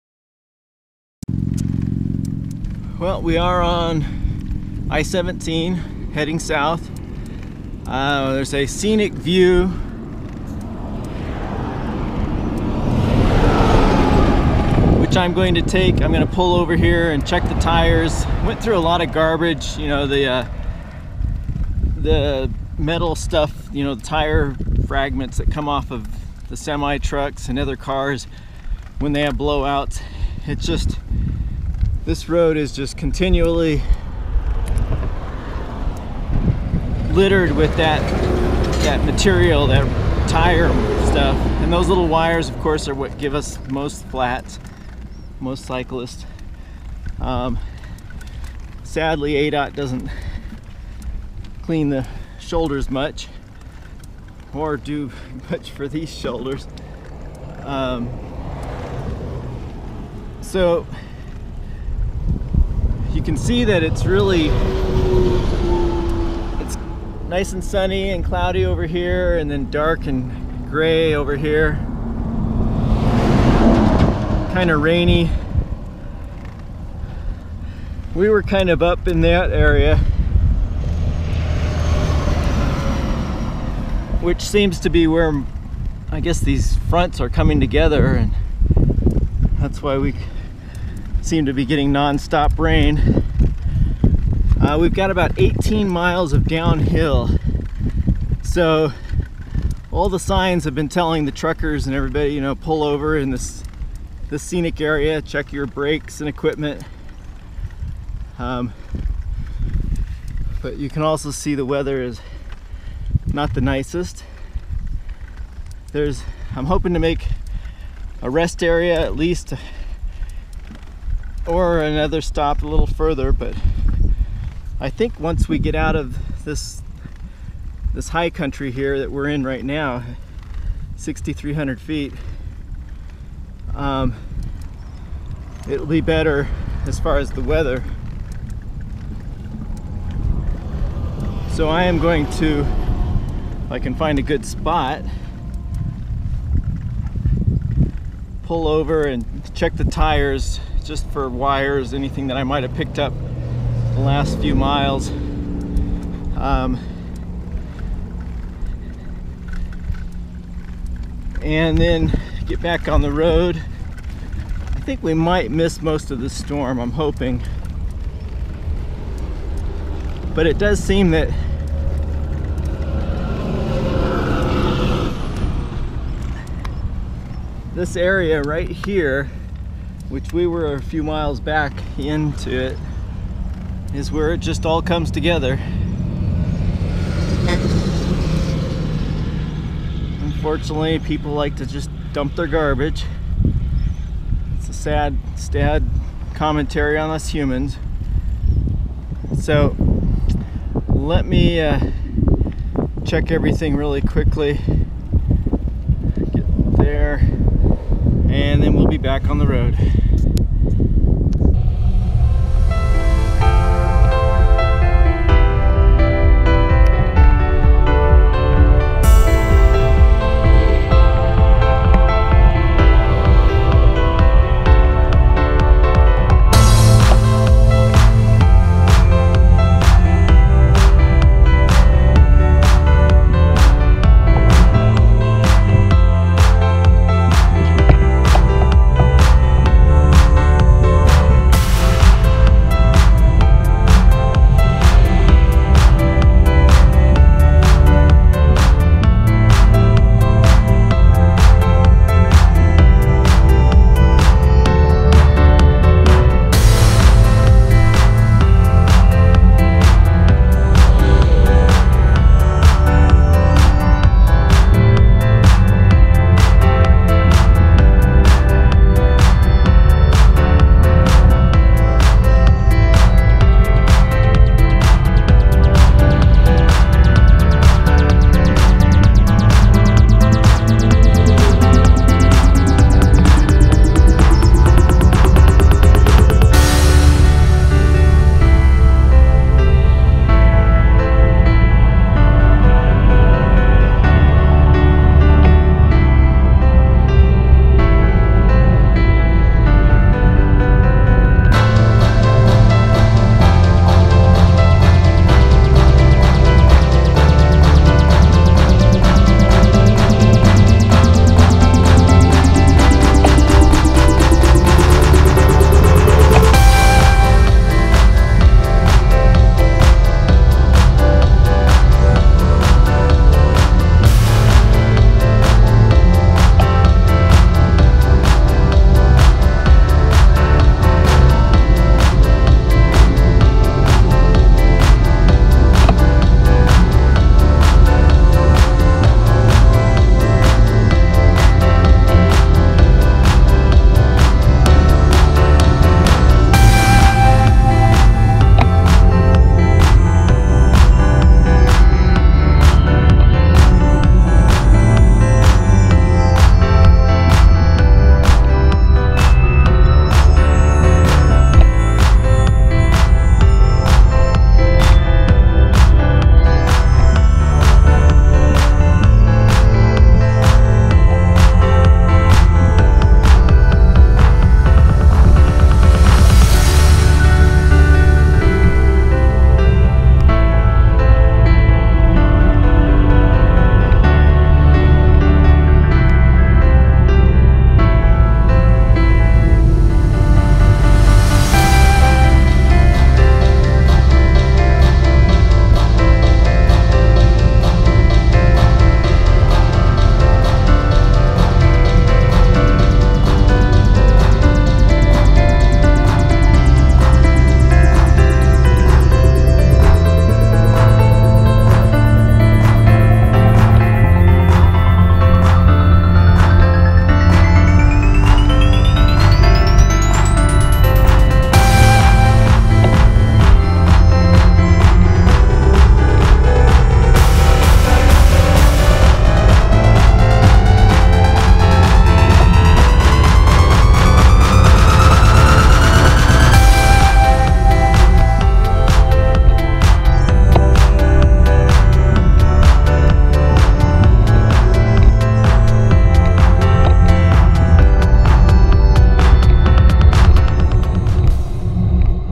#163 - Leaving the High Country
Stormy weather, road debris, and lower elevation.
Please support your local animal rescue shelter.